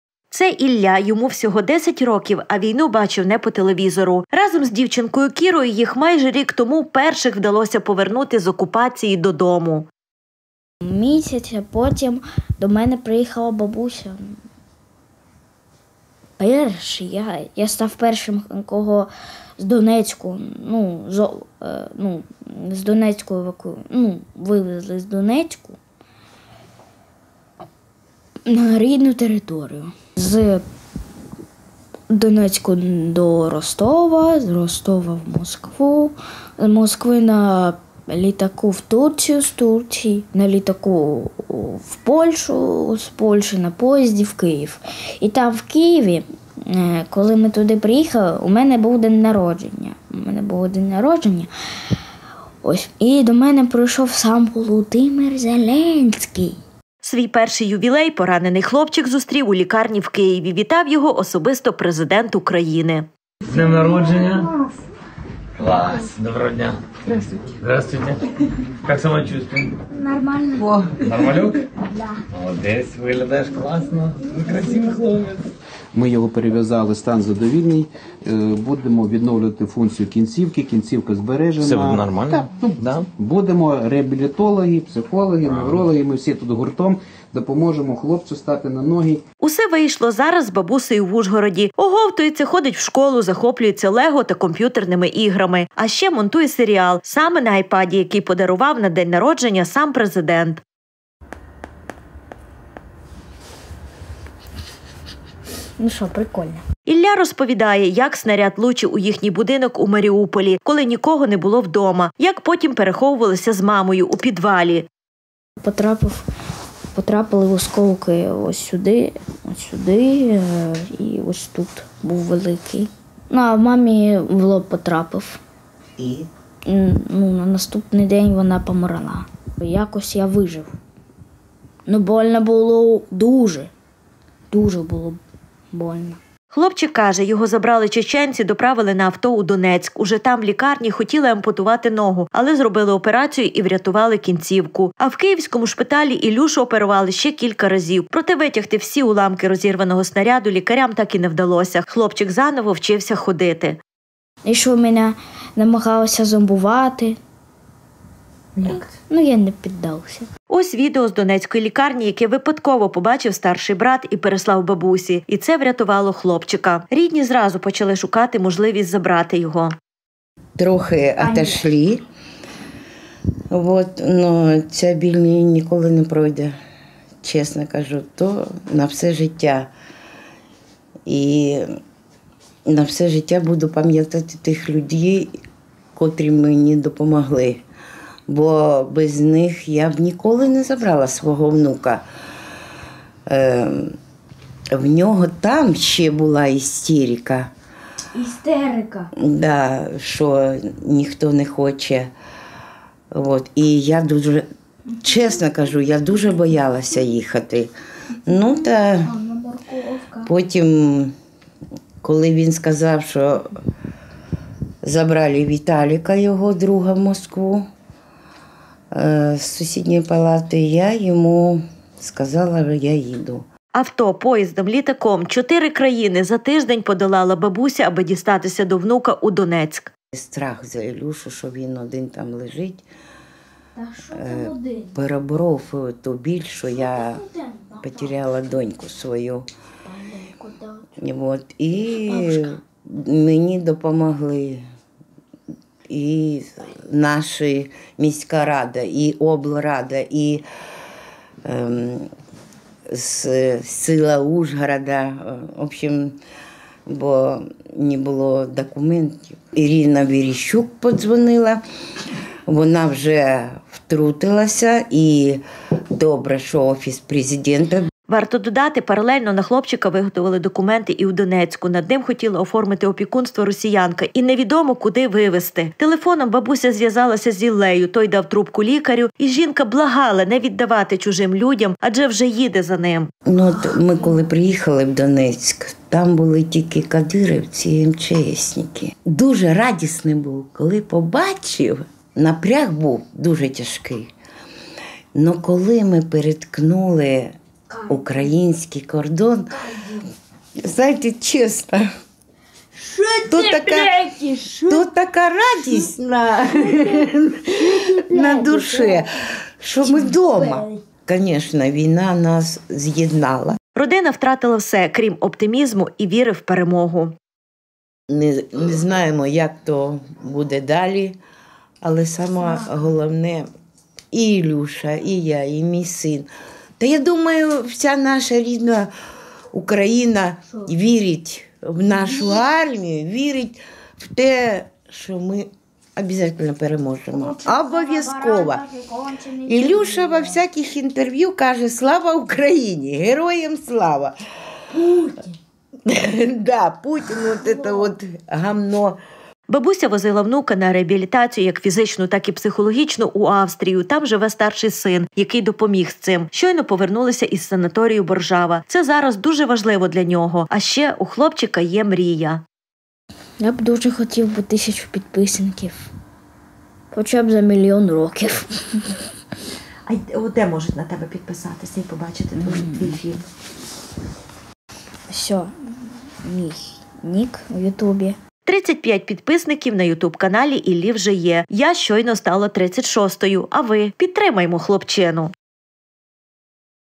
Шанс на життя!
Шанс на життя отримав Ілля з Маріуполя! Після того, як будинок родини зруйнували, а мама загинула, пораненого 10-річного хлопчика вивезли у Донецьк,  хотіли всиновити та вивезти в росію. Відео ворожого каналу про нього у лікарні випадкво потрапило до рук бабусі, яка проживає в Ужгороді,  і  жінка зразу стрімголов почала шукати можливість забрати онука! І – все вдалося! Іллюша та ще одна дівчинка Кіра  – перші вивезені діти з окупованого Донбасу ! Це Ілля – йому всього 10 років, а війну бачив не по телевізору! Разом з дівчинкою Кірою їх майже рік тому перших вдалося повернути з окупації додому!  Свій перший ювілей  поранений хлопчик зустрів у лікарні в  Києві, вітав його  особисто президент України. Усе вийшло! Зараз з баусею в Ужгороді – оговтується, ходить у школу, захоплюється лего та  комп’ютерними іграми.  А ще монтує серіал – саме на айпаді, який подарував на день народження сам президент. Ілля розповідає, як снаряд влучив у їхній будинок у Маріуполі, коли нікого не було вдома, як потім переховувалися з мамою у підвалі .Хлопчик каже, його забрали чеченці, доправили на авто у Донецьк. Уже там в  лікарні хотіли ампутувати ногу, але зробили операцію і врятували кінцівку.  Ось відео з донецької лікарні, яке випадково побачив старший брат і переслав бабусі, і це врятувало хлопчика! Рідні зразу почали шукати можливість забрати його ! Авто, поїздом, літаком  - 4 країни за тиждень подолала бабуся, аби дістатися до внука у Донецьк. Варто додати, паралельно на хлопчика виготвили документи і у Донецьку, над ним хотіла оформити опікунство росянка і невідомо куди вивезти! Телефоном бабуся зв’язалася з Іллею, той дав трубку лікарю – і жінка благала не віддавати чужим людям, адже вже їде за ним!  Родина втратила все, крім оптимізму і віри в Перемогу! Бабуся возила внука на реабілітацію- як фізичну, так і психологічну, у Австрію, там живе старший син, який допоміг з цим. Щойно повернулися із санаторію Боржава. Це – зараз луже важливо для нього! А ще у хлопчика є мрія!  А ви? Підтримаймо хлопчину!